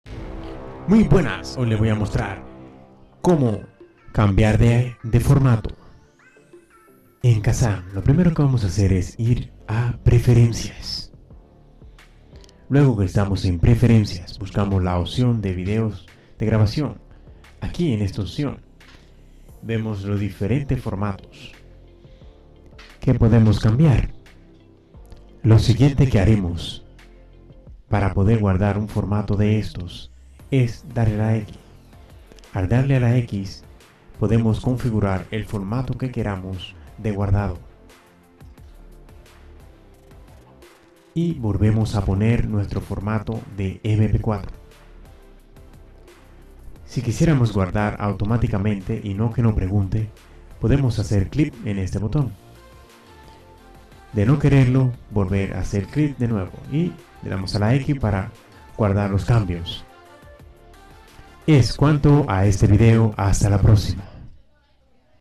Como cambiar el formato de guardado d video de Kazam
Mp4, avi. etc podras cambiar para que tu capturador de pantalla grabe en otro formato los videos que haces de tu escritorio.